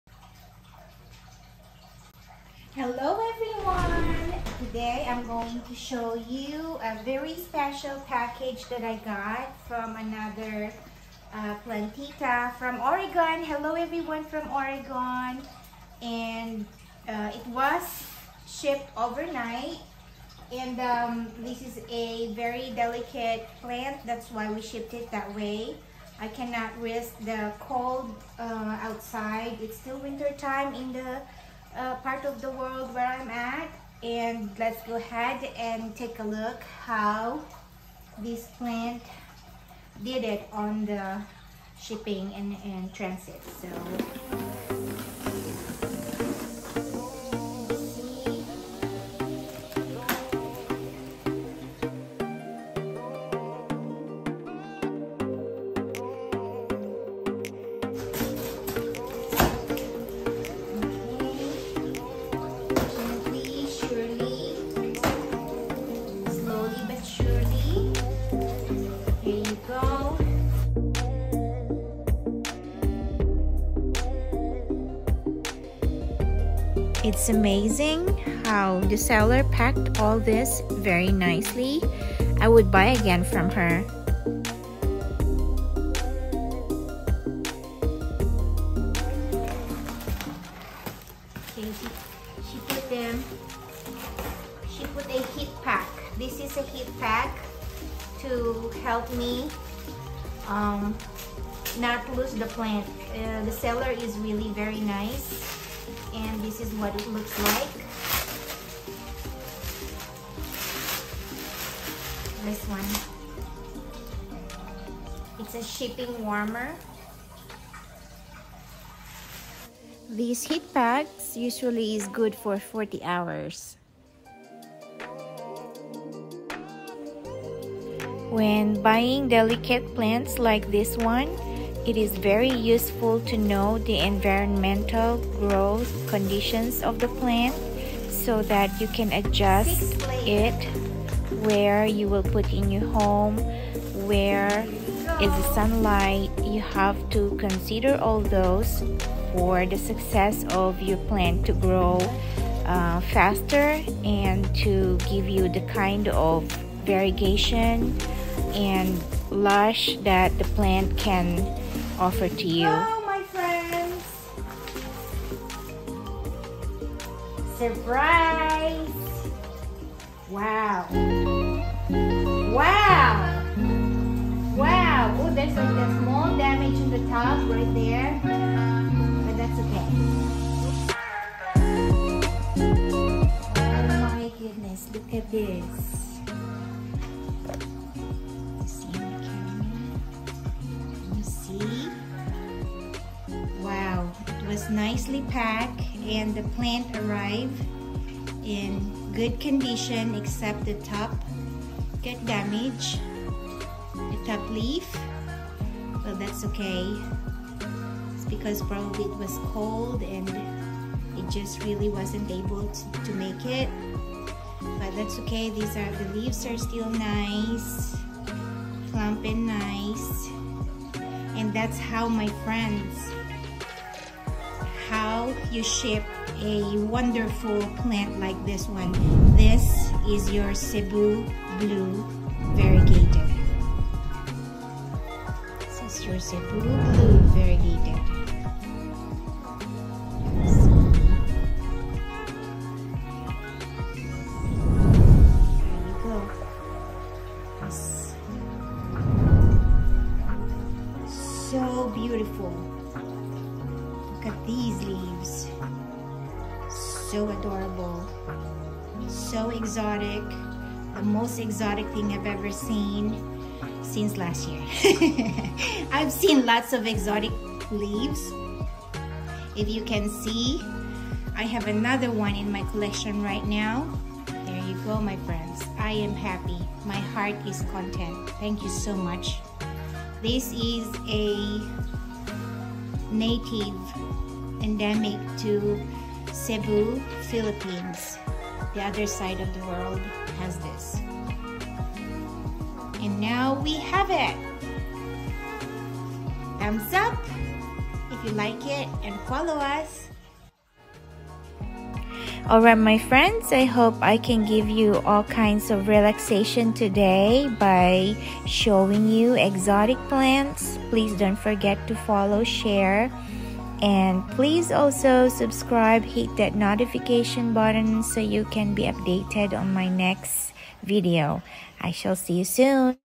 Cebu Blue Variegated Ghost - Epipremnum Pinnatum Cebu Blue Variegated Ghost Super Rare Aroid of 2022
Epipremnum Pinnatum Cebu Blue Ghost - I am one of the luckiest Plant Mama to own a gorgeous plant like this one.
Remember those days where we were rushing to big buck stores to buy our most favorite Cebu Blue? Well here is the variegated one! And it is indeed as astounding as we were imagining!